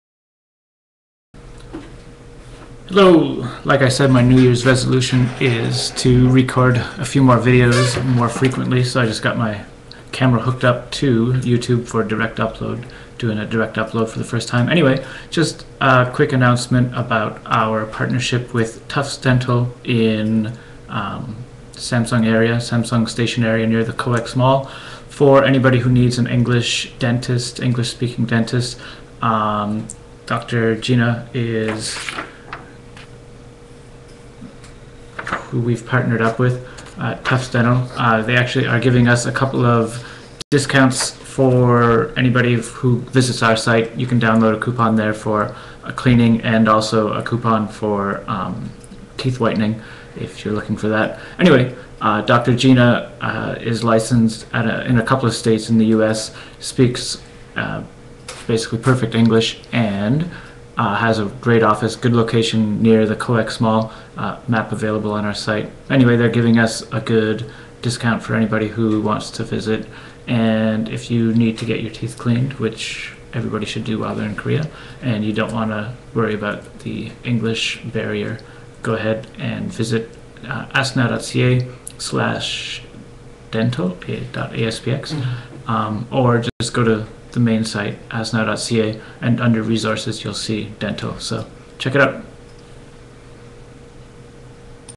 ASK Now and Tufts Dental...English Dentist in Seoul
Need an English-speaking dentist in Seoul? Nobody better than Dr. Gina.